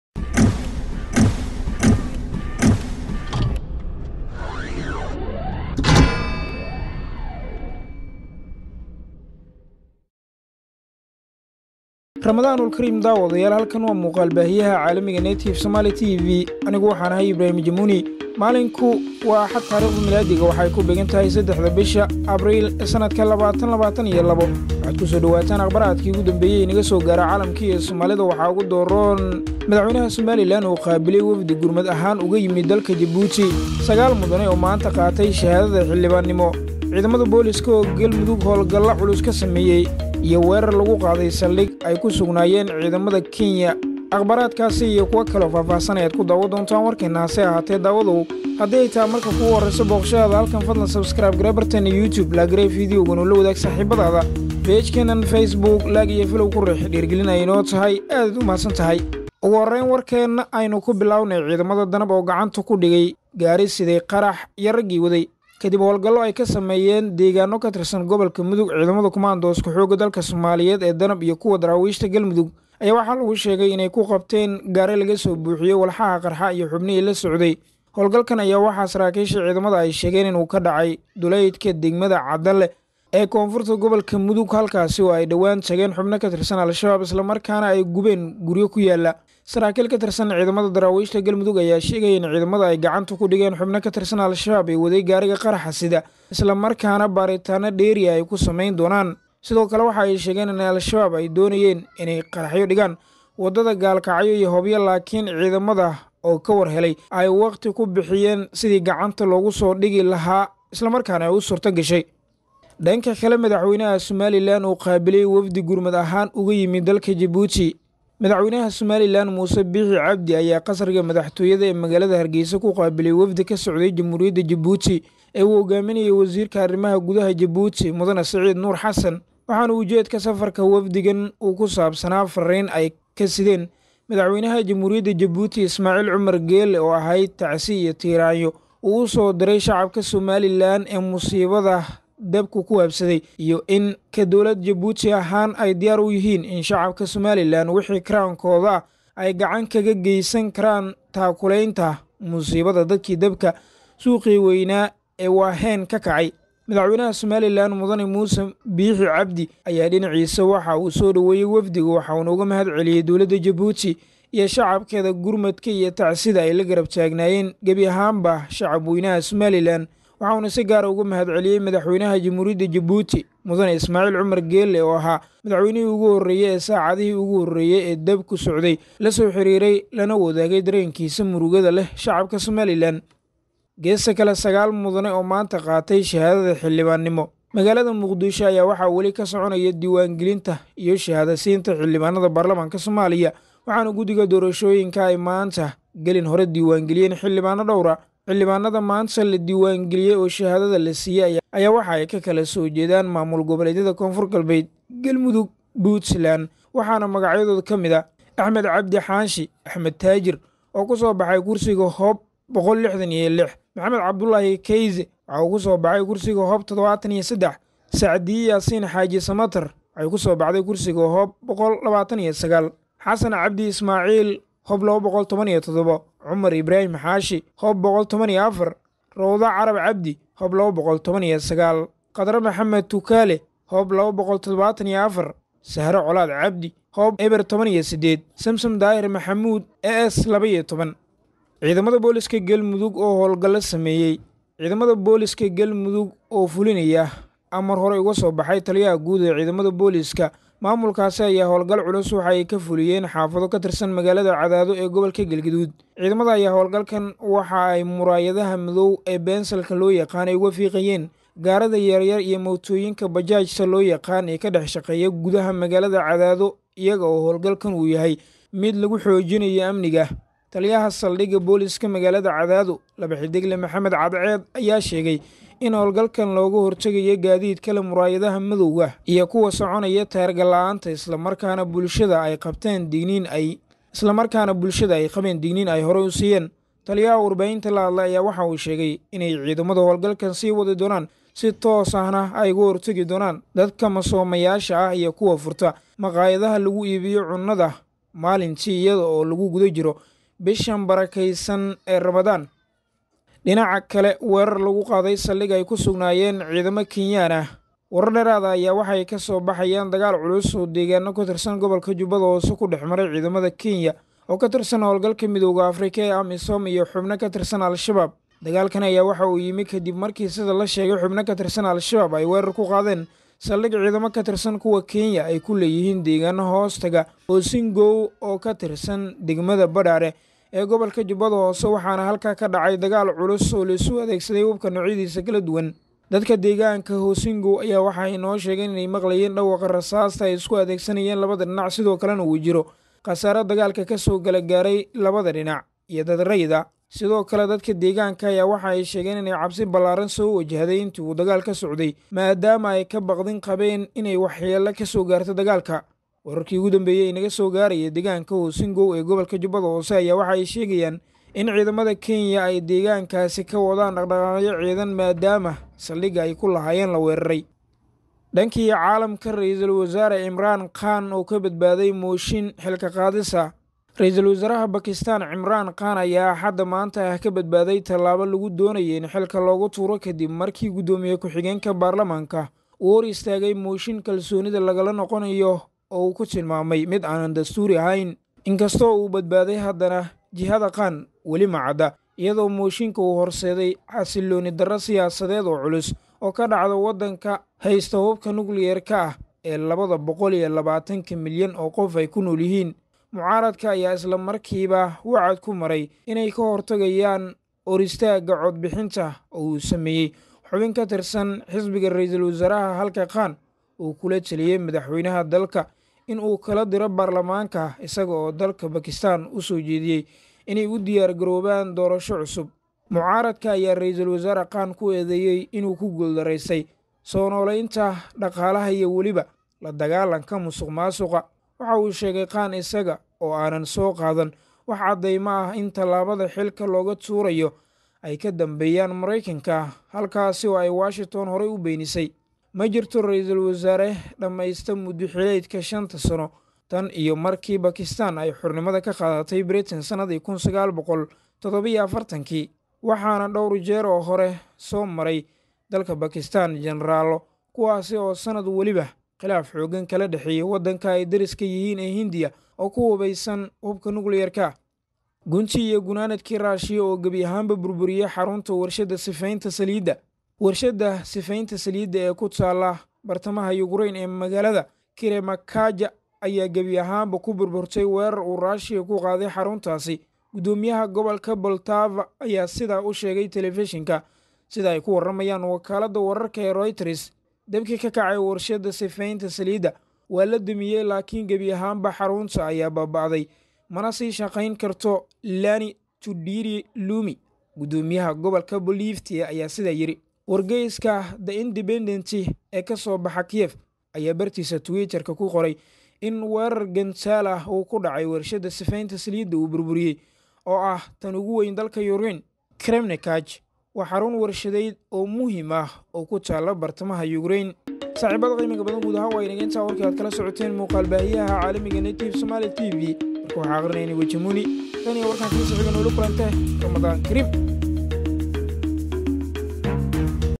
DEG DEG 3 4 2022 WERAR ASKAR LAGU DILAY OO KA DHACAY,XUBNO AL SHABAB OO LASOO QABTAY,MADAX BOOQASHO
DEG DEG 3 4 2022 WERAR ASKAR LAGU DILAY OO KA DHACAY,XUBNO AL SHABAB OO LASOO QABTAY,MADAX BOOQASHO KU TIMID SOMALILAND,IMRAN KHAN OO SIKTAY,RUUSHKA OO BURBURIYEY WARSHAY SALIIDA,9 XILIBAAN OO SHAHAADADA MAANTA QAATAY.